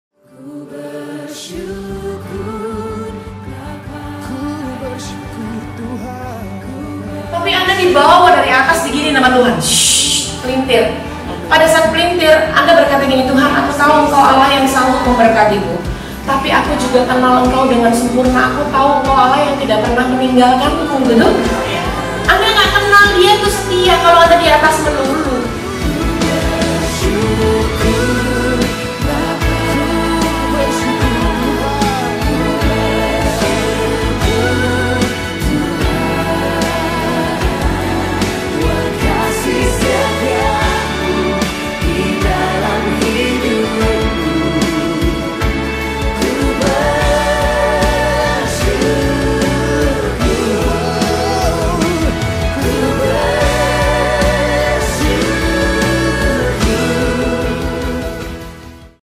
TEASER IBADAH SYUKUR ULANG TAHUN KE-2 XAVIER MARKS INDONESIA - with Ibu Gina Dharmawan
IBADAH SYUKUR
ULANG TAHUN KE-2
XAVIER MARKS INDONESIA
KAMIS, 21 FEBRUARI 2019
PK. 10AM - SELESAI
@ XMI Hall, Xavier Marks Indonesia
Pembicara : Ibu GINA DHARMAWAN
(trainer, motivator, business coach dari Jakarta)
Mengundang seluruh keluarga besar XAVIER MARKS untuk hadir. Demi kenyamanan bersama harap hadir 30 menit sebelum acara dimulai
Selamat Ulang tahun ke-2 XAVIER MARKS INDONESIA!